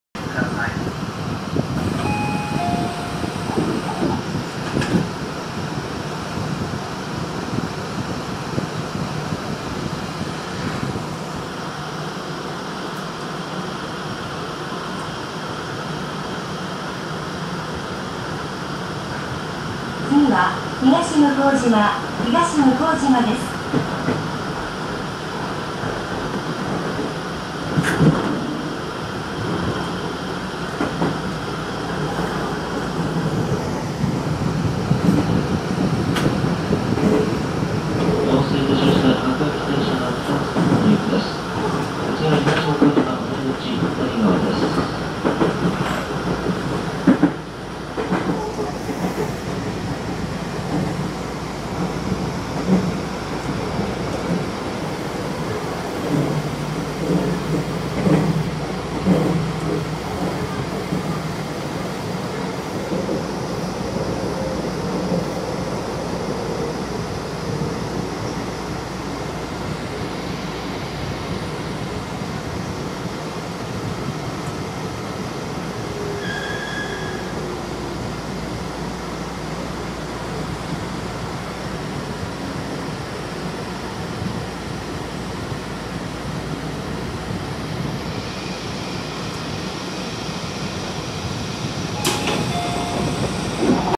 東武伊勢崎線 曳舟～東向島駅 前面展望
曳舟～東向島駅までの前面展望です
すこしだけメーターも写っています